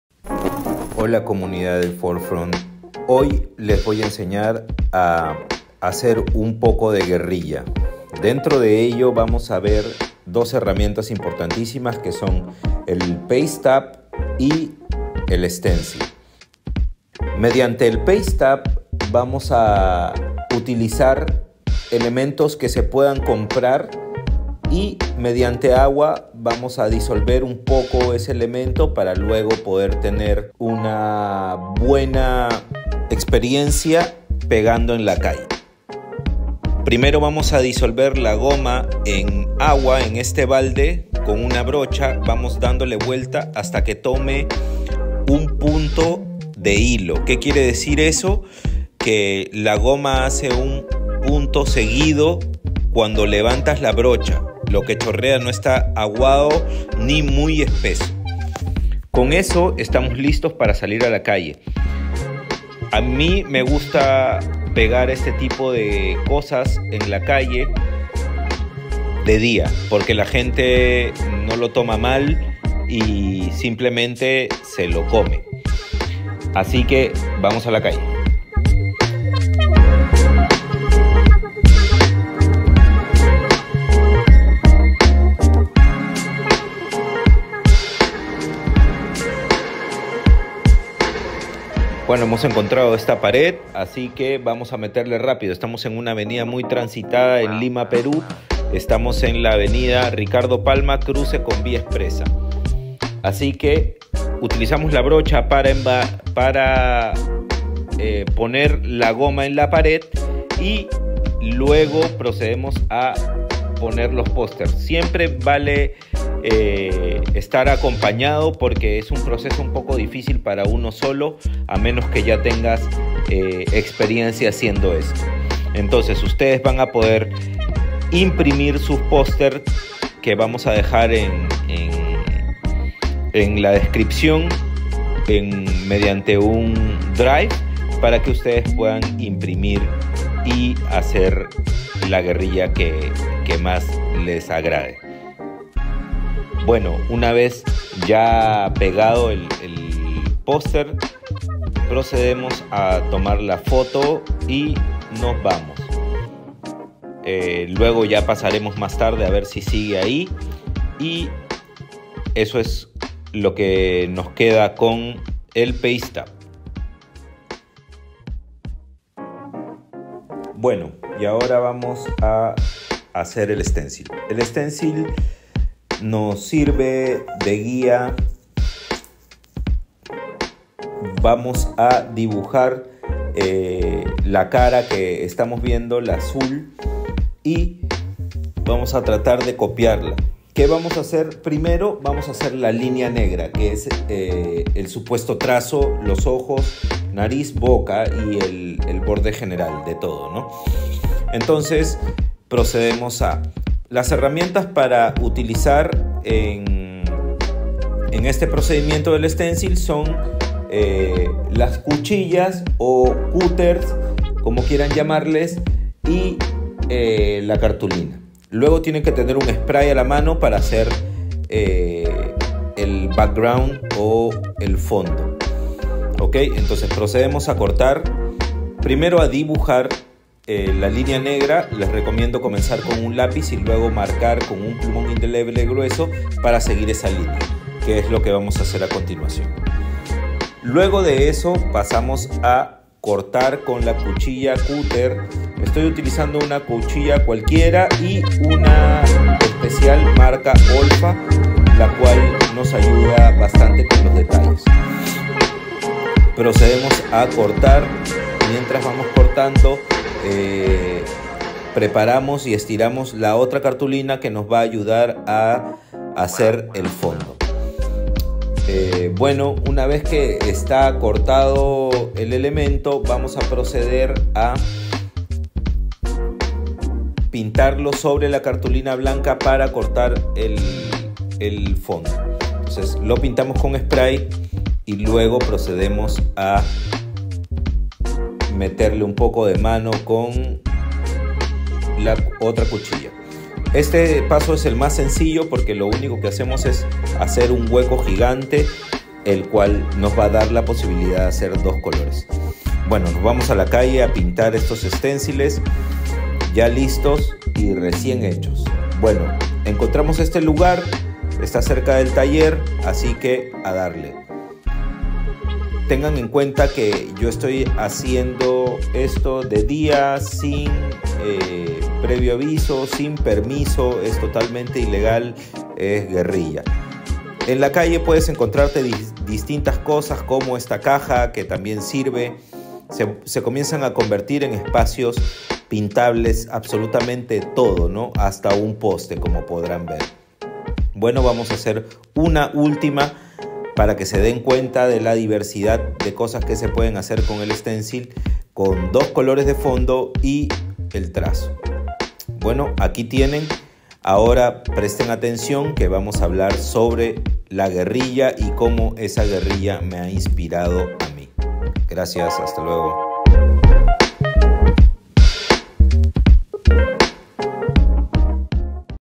Entes - The guerrilla of Caras Latinas
W3CR Resident Creator Entes dives into guerrilla, an approach to art that involves often anonymous interventions in public spaces, and how he's integrating this into his NFT project, Caras Latinas.

Resident Creator Entes is a multi-disciplinary graffiti & street artist of more than 2 decades, hailing from Peru. He has exhibited internationally, incl. Museum of Graffiti of Miami, The Straat Museum of Amsterdam, and Contemporary Art Museum of Bogota. He is currently expressing the rich dynamism of Afro-Peruvian culture via his NFT project, Caras Latinas (”Latam Faces”).